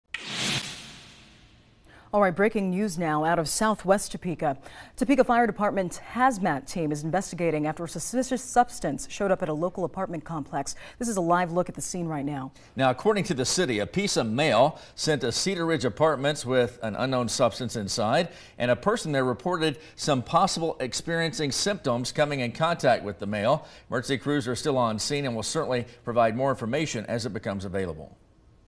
Hazmat investigates 'suspicious package' in southwest Topeka
A Hazardous Materials Response team is in southwest Topeka responding to a call involving a "suspicious substance".